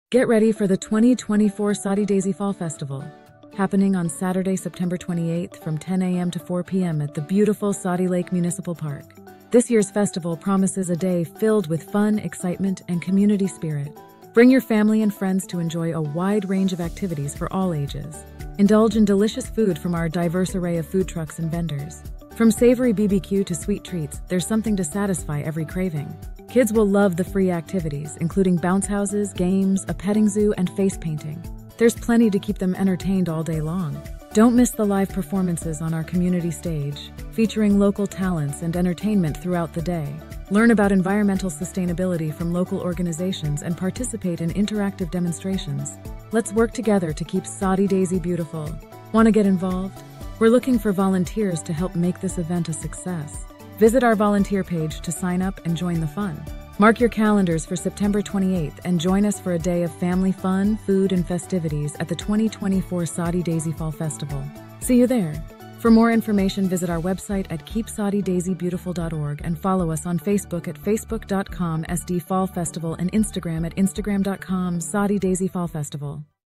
Join Us at the 2024 Soddy-Daisy Fall Festival! 🍂 | Keep Soddy-Daisy Beautiful
Get ready for the biggest community event of the year! The 2024 Soddy-Daisy Fall Festival is happening on September 28, 2024, from 10:00 AM to 4:00 PM at the beautiful Soddy Lake Municipal Park, 197 Durham St, Soddy-Daisy, TN 37379.

🎉 What to Expect:
✨ Over 100 Vendors featuring local crafts, food, and unique products
🎶 Live Entertainment throughout the day with amazing performers
🎈 Free Family Activities including bounce houses, face painting, and a soft play zone
🎁 Exciting Giveaways with fantastic prizes announced by our MC from J103
🐴 Horse Carriage Rides provided by Chattanooga Carriage Company

Join us for a day filled with fun, food, and community spirit, all while supporting Keep Soddy-Daisy Beautiful's mission to enhance our local environment and public spaces.

📅 Save the Date: September 28, 2024
📍 Location: Soddy Lake Municipal Park, 197 Durham St, Soddy-Daisy, TN 37379

Vendor Registration: If you want to be part of this incredible event, register

📢 Don’t miss out on this fantastic event! Mark your calendars, invite your friends, and join us for a day of fun at the 2024 Soddy-Daisy Fall Festival! See you there!